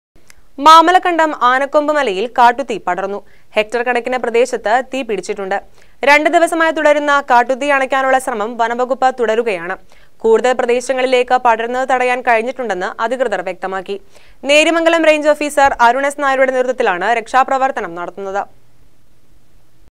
മാമലകണ്ടം ആനക്കൊമ്പ് മലയില്‍ കാട്ടുതീ പടര്‍ന്നു.
മാമലകണ്ടം ആനക്കൊമ്പ് മലയില്‍ കാട്ടുതീ പടര്‍ന്നു.ഹെക്ടര്‍കണക്കിന് പ്രദേശത്ത് തീ പിടിച്ചിട്ടുണ്ട്.രണ്ട് ദിവസമായി തുടരുന്ന കാ്ട്ടുതീ അണക്കാനുള്ള ശ്രമം വനംവകുപ്പ് തുടരുകയാണ്.